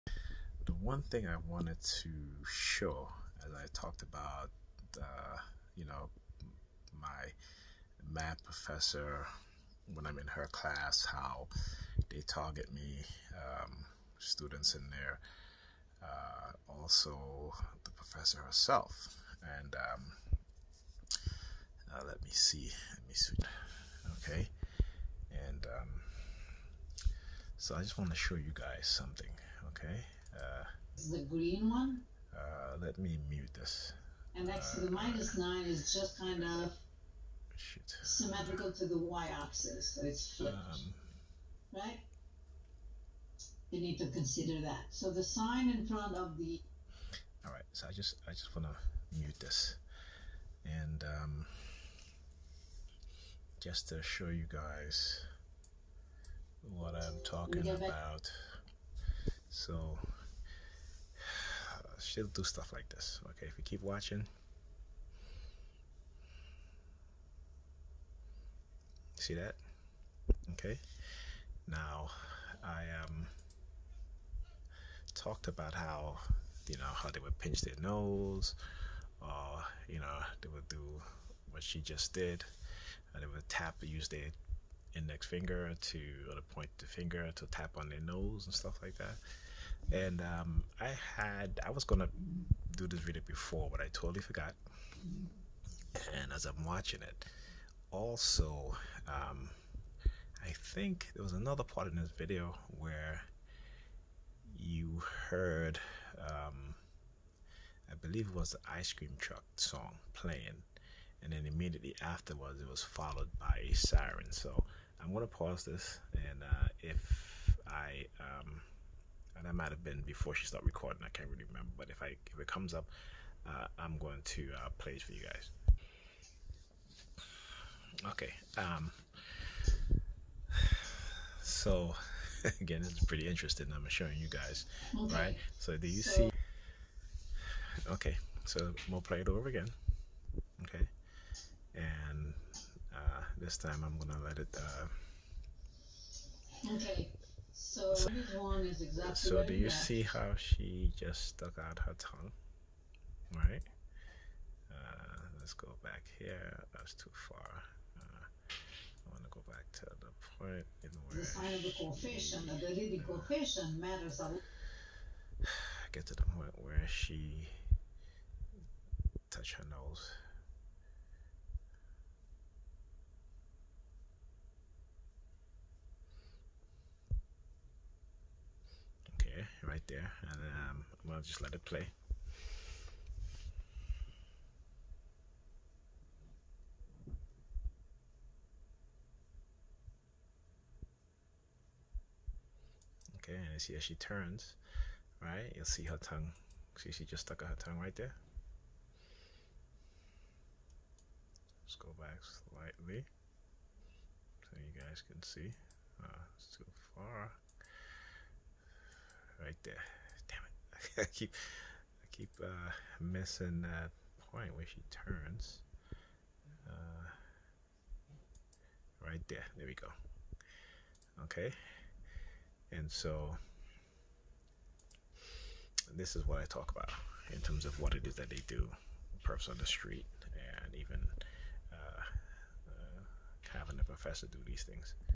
The targeted Individual Program 10_13_2020 Part 3
Documenting my targeting in this criminal Coercive Persuasion Program, Non Consensual Human Experimentation Program and Illegal Micro-Chipping Program for the purpose of Remote Neural Monitoring.